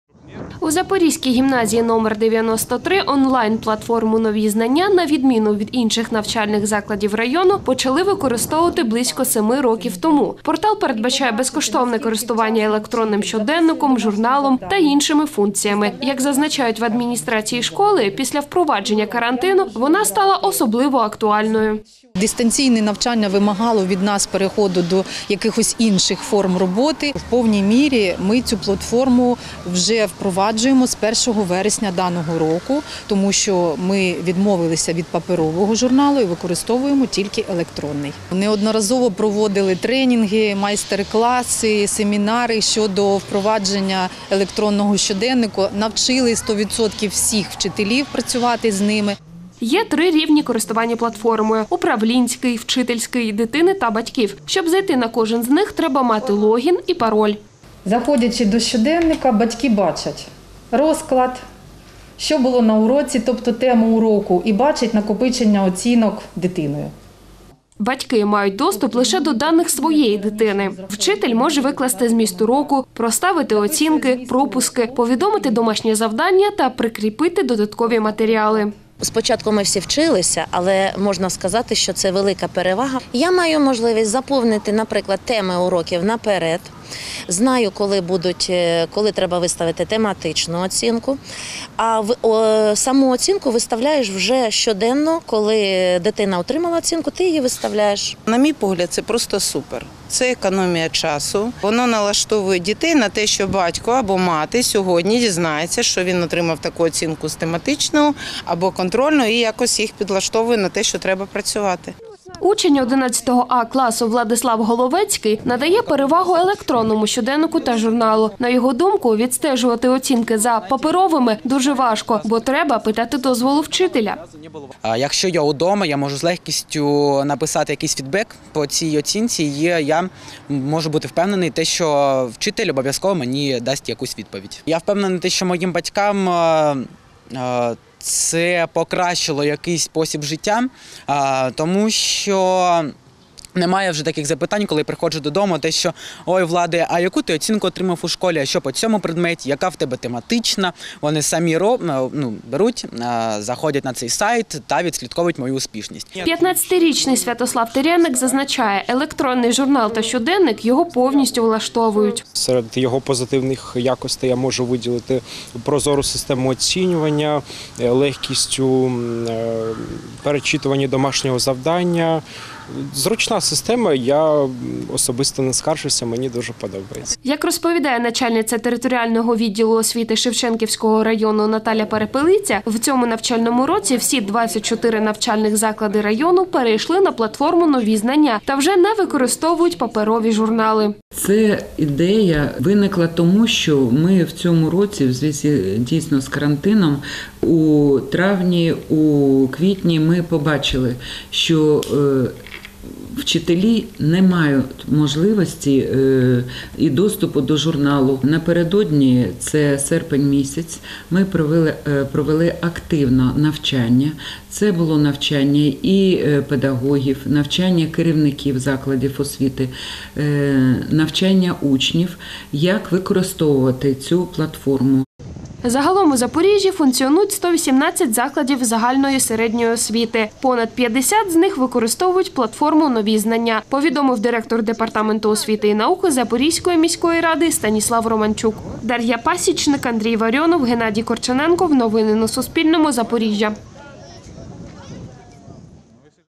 Електронне оцінювання успішності учнів | Новини | 21.10.2020
Всі школи Шевченківського району Запоріжжя перейшли на електронне оцінювання успішності учнів - про це повідомила Наталя Перепелиця, начальниця територіального відділу освіти у Шевченківському районі. З початку вересня у районних закладах освіти масово впроваджують онлайн-платформу "Нові знання".